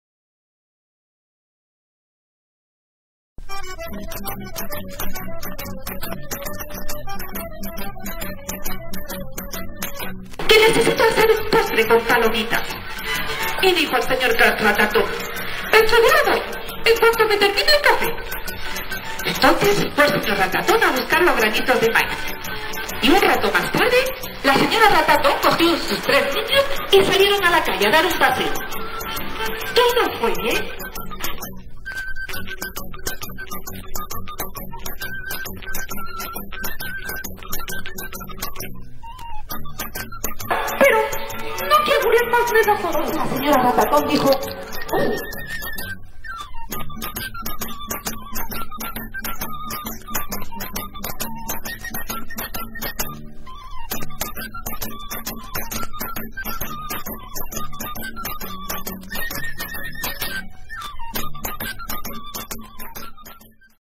CUENTACUENTOS Mª ANGUSTIAS TORO
Actuación en el municipio de Armilla en verano de 2009